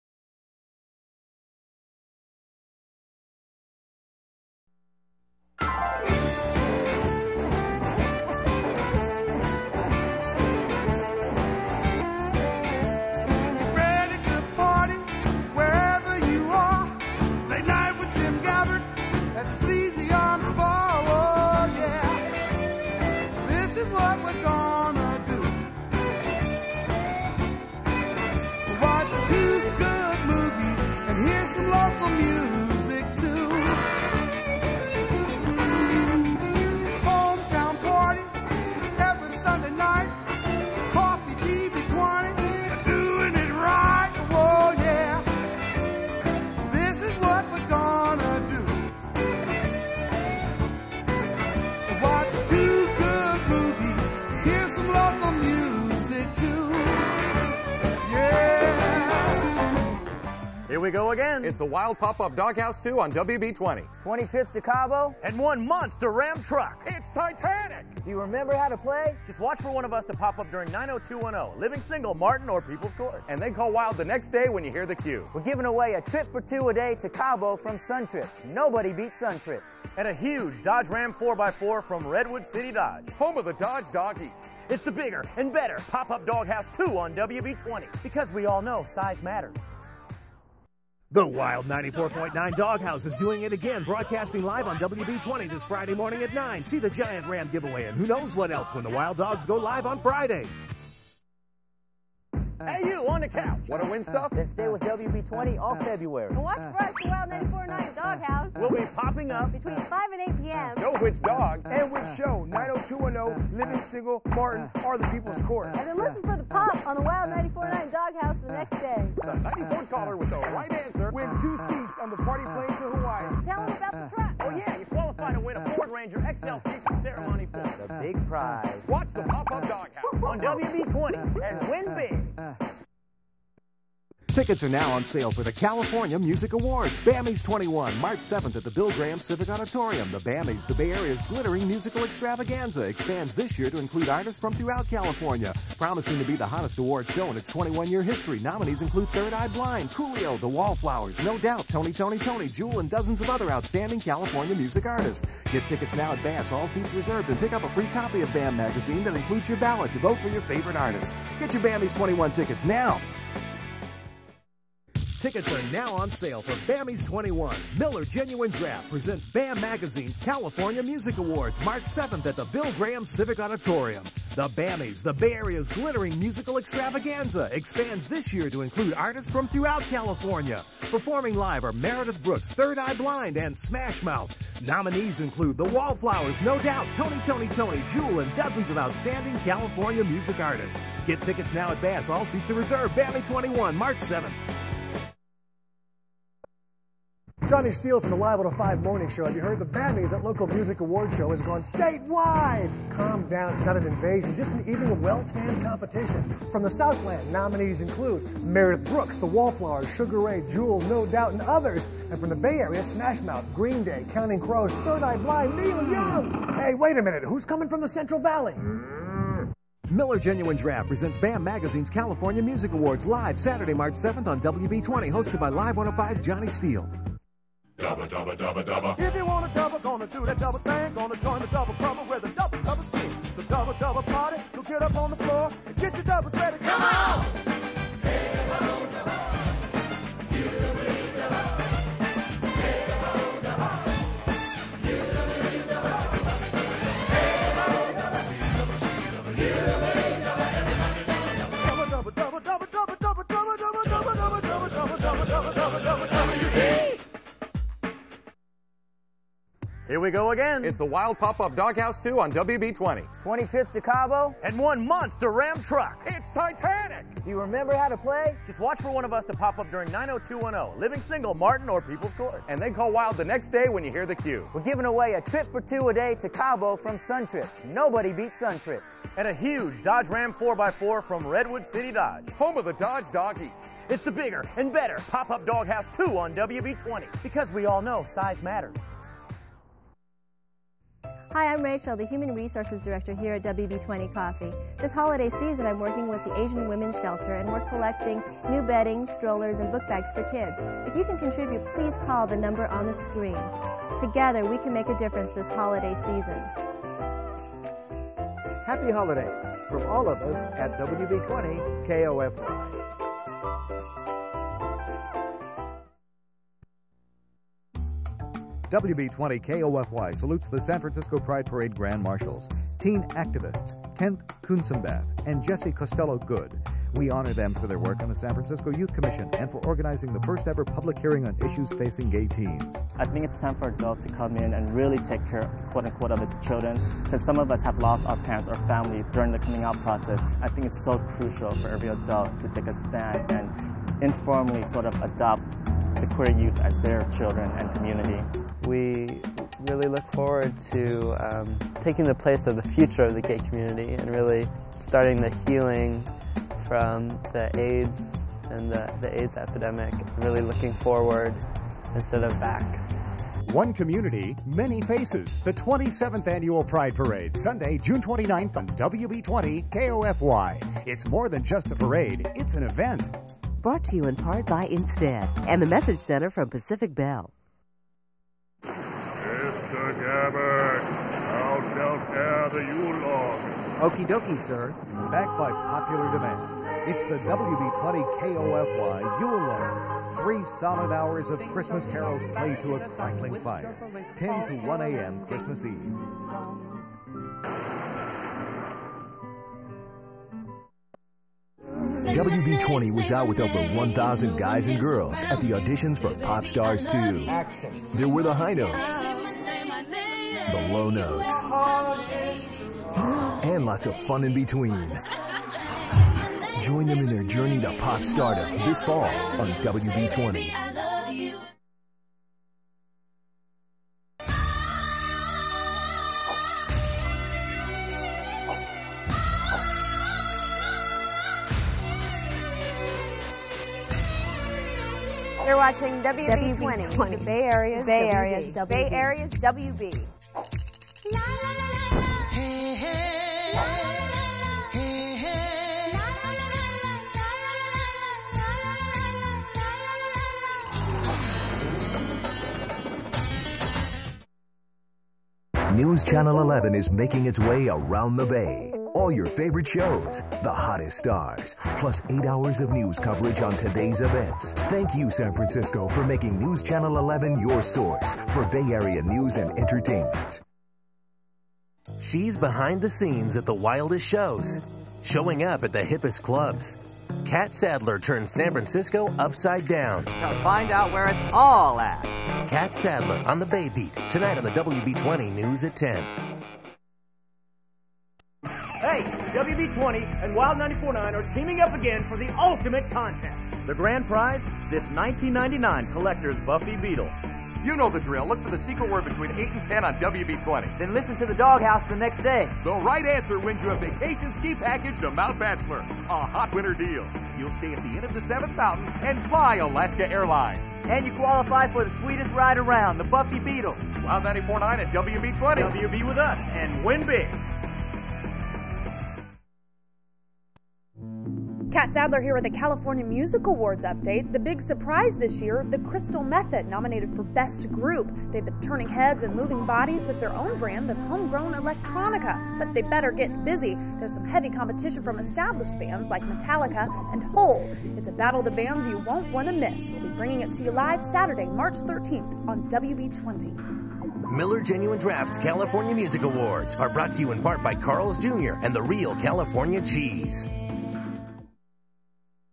Jeff Fisher Archive 6
This archive (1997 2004) is from Jeff Fisher, an multi Emmy award winner editor. I worked with Jeff at KOFY "TV 20", WB in San Francisco and shot a lot for Jeff. He always made my footage look good. Jeff was also an award winning producer specializing in promotions. Here is a very small sample of his large body of work.
TV20 KOFY Promos edited by Jeff Fisher